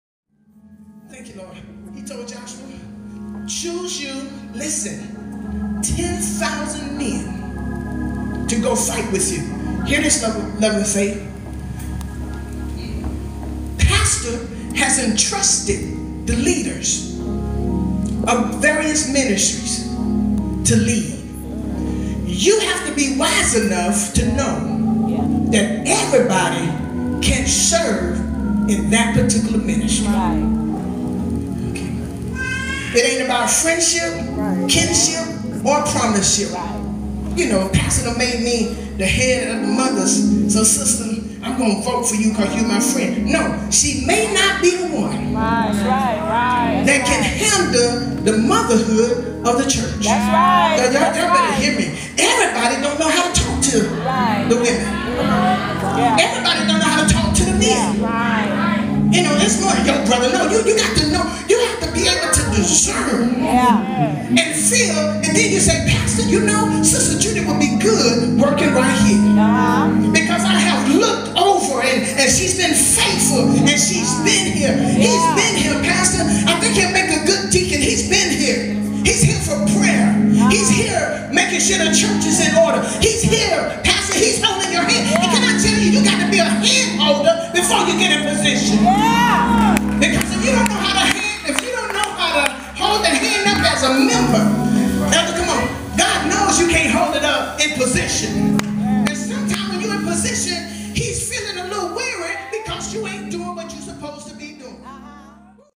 Evangelist Judy Mandrell - "The Power of the Hands Holders" (Clip 2)
This powerful message was preached at Love & Faith Community Church in Tallahassee, Florida, during the Kingdom Servanthood Celebration for Pastor Lamar Simmons.